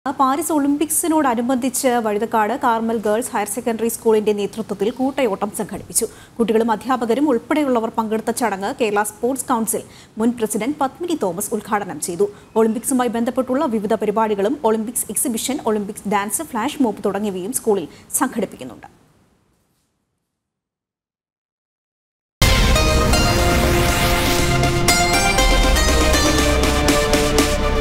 പാരീസ് ഒളിമ്പിക്സ്: വഴുതക്കാട് കാർമൽ ഗേൾസ് സ്കൂളിന്‍റെ നേതൃത്വത്തിൽ കൂട്ടയോട്ടം സംഘടിപ്പിച്ചു
പാരീസ് ഒളിമ്പിക്സിനോട് അനുബന്ധിച്ച്  വഴുതക്കാട് കാർമൽ ഗേൾസ് ഹയർ സെക്കണ്ടറി സ്കൂളിന്‍റെ നേതൃത്വത്തിൽ  കൂട്ടയോട്ടം സംഘടിപ്പിച്ചു. കുട്ടികളും അധ്യാപകരും ഉൾപ്പെടെയുള്ളവർ പങ്കെടുത്ത ചടങ്ങ്   കേരള  സ്പോർട്സ് കൗൺസിൽ മുൻ പ്രസിഡന്‍റ് പത്മിനി തോമസ് ഉദ്ഘാടനം ചെയ്യ്തു. ഒളിമ്പിക്സുമായി ബന്ധപ്പെട്ടുള്ള വിവിധ പരിപാടികളും. ഒളിമ്പിക്സ് എക്സിബിഷൻ, ഒളിമ്പിക്സ് ഡാൻസ്, ഫ്ലാഷ് മോബ് തുടങ്ങിയവും സ്കൂളിൽ സംഘടിപ്പിക്കുന്നുണ്ട്..